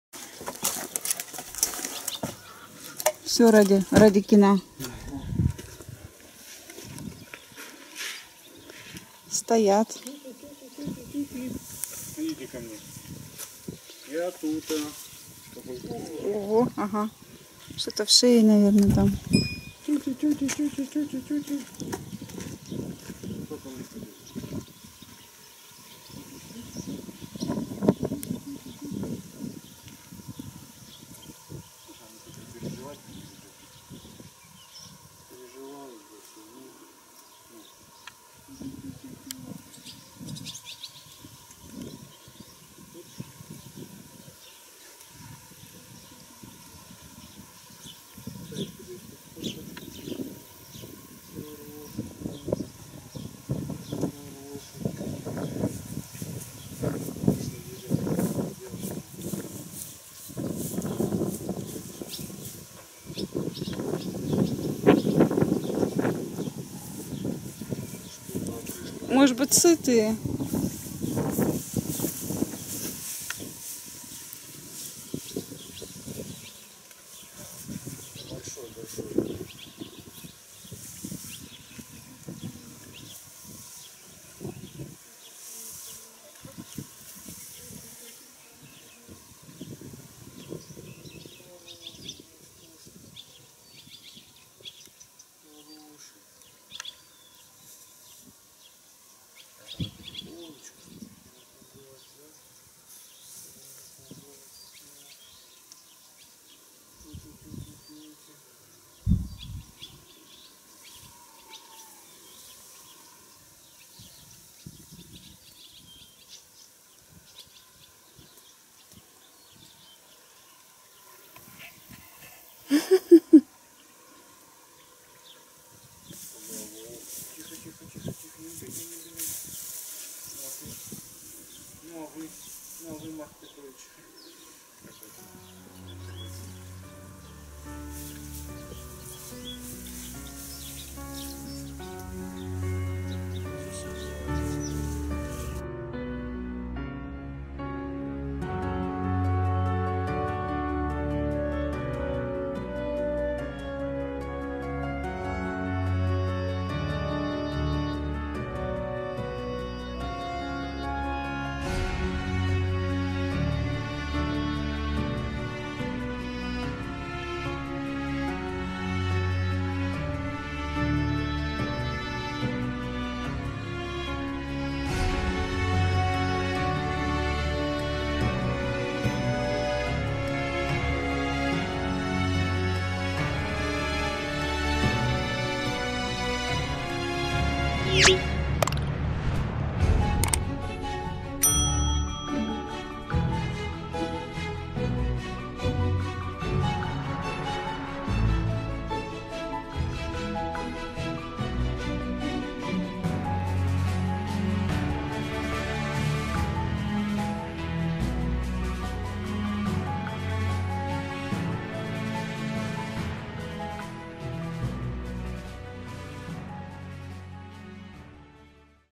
Как австралийские страусы Эму ведут себя в Сибири, интересные отношения к хозяину. Истории из жизни
Нашёл недавно АРХИВНУЮ СЪЁМКУ это настоящие – истории из жизни. Однажды в нашем хозяйстве были красивые австралийские страусы Эму. Страусы Эму ведут себя очень интересно. К хозяину относятся доверительно, хотя сами по себе очень пугливы и глупы. Эму не агрессивные, в отличии от африканских. Страус Эму давно уже живёт в Сибири и к нашему климату немного привык.

Почему мы не стали держать Эму читайте здесь:
Часть 1